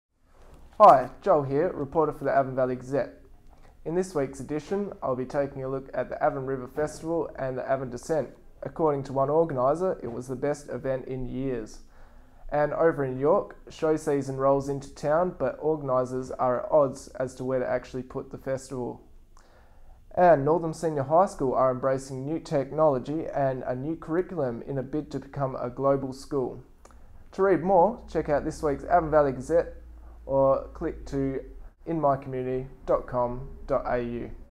Avon Valley Gazette - 13-08-11
Journalist Joel Kelly with some of the main stories appearing in tomorrow's issue of the Avon Valley Gazette (13-08-11).